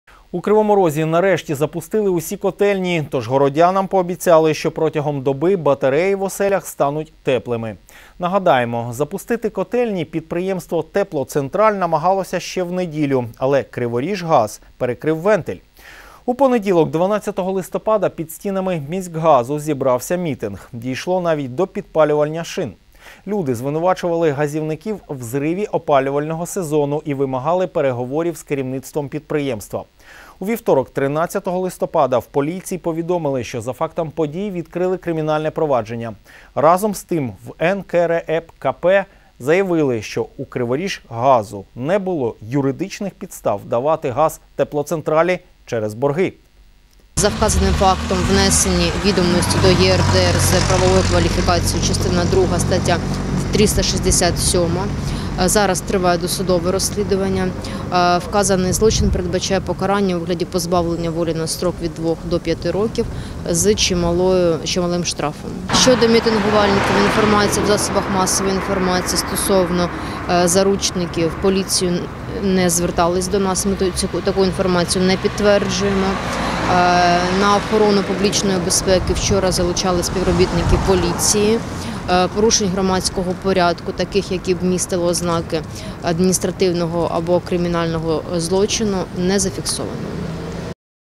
У Кривому Розі нарешті запустили всі котельні, але поліція вже відкрила кримінальне провадження
У Кривому Розі нарешті запустили усі котельні, тож городянам пообіцяли, що протягом доби батареї в оселях стануть теплими. Нагадаємо: запустити котельні підприємство "Теплоцентраль" намагалося ще в неділю, але "Криворіжгаз" перекрив вентиль. У понеділок, 12 листопада, під стінами міськгазу зібрався мітинг, дійшло навіть до підпалювання шин. Люди звинувачували газівників в зриві опалювального сезону й вимагали переговорів з керівництвом підприємства. У вівторок, 13 листопада, в поліції повідомили, що за фактом подій відкрили кримінальне провадження. Разом з тим, в НКРЕКП заявили, що у "Криворіжгазу" не було юридичних підстав давати газ "Теплоцентралі" через борги.